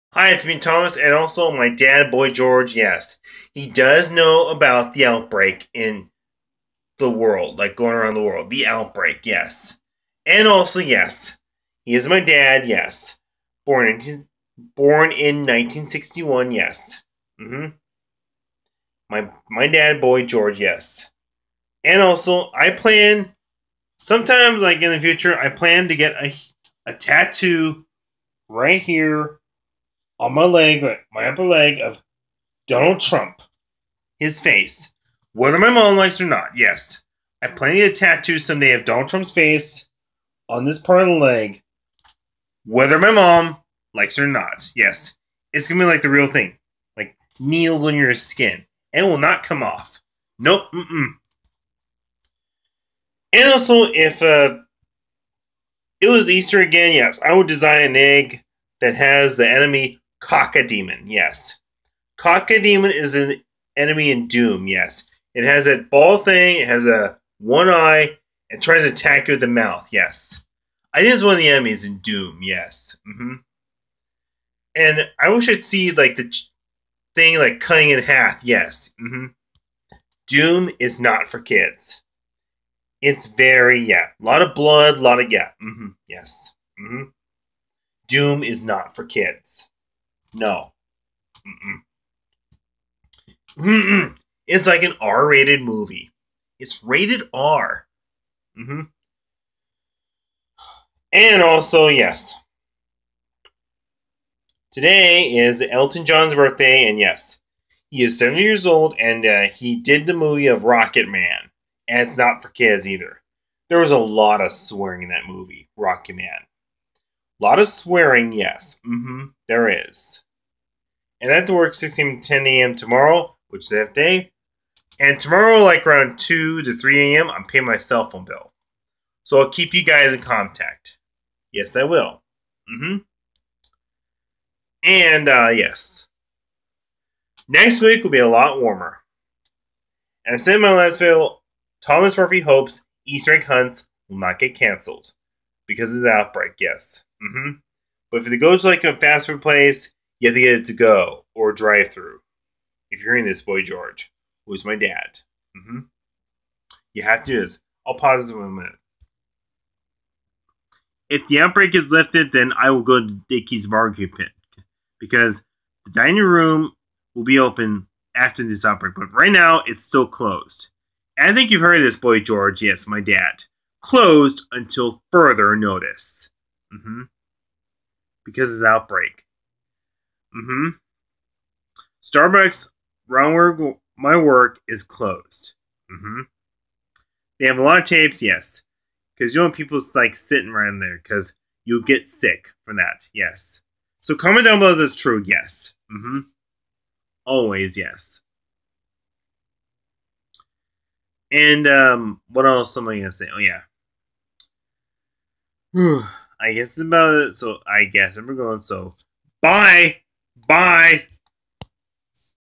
Thomas Murphy's dad Boy George knows about the outbreak. Closed until further notice.
Thomas Murphy's dad Boy George knows about the outbreak. Closed until further notice. The outbreak is worldwide. He is born in 1961. Thomas plans to get a tattoo of Donald Trump's face on his upper left leg whether his mom likes it or not. It will be the real thing needle going into skin and it will not come off if touched. If it's Easter again, Thomas will design an egg and it will be a cacodemon from the videogame DOOM which is one of the enemies. The Cacodemon has one eye and tries to attack with the big mouth. DOOM is not for kids. Lot of blood. Today is elton john's birthday and he is 73 years old and he did the movie Rocketman which is not for kids either. There is a lot of swearing in Rocketman. Thomas will pay his bill of $40 to metro. The last video is Thomas Murphy hopes no easter egg hunts get cancelled. If you want to eat out, it will be the drive-thru or to-go. If the outbreak is over, Thomas will go to Dickey's BBQ because the dining room will be open but right now it is closed. Thomas's dad Boy George has heard of this:closed until further notice. Starbucks is closed and it has to re-open in Mid-April. Next video is March 26.